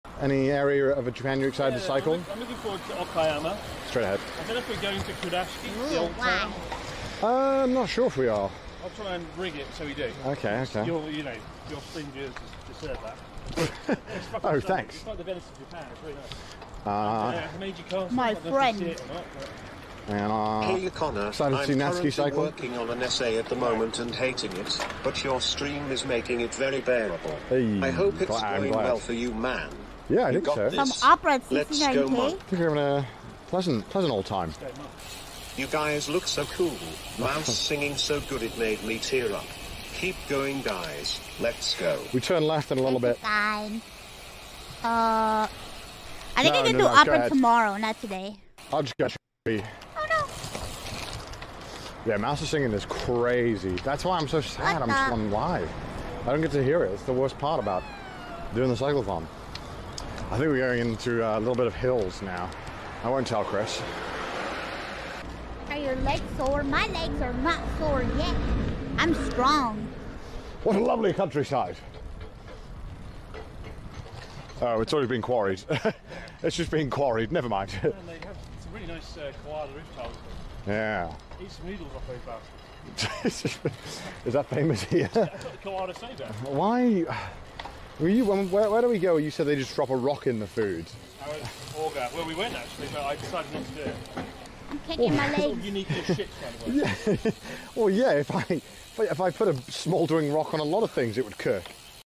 CDawgVA Makes Ironmouse Blushes Praising her Singing
Thumbnail Art by: lamidraws